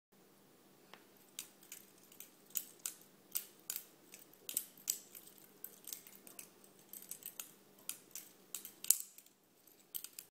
Darrel Ralph DDR Venturi VIII Balisong
Still trying to get used to this knife.

It's fast because of the bearings.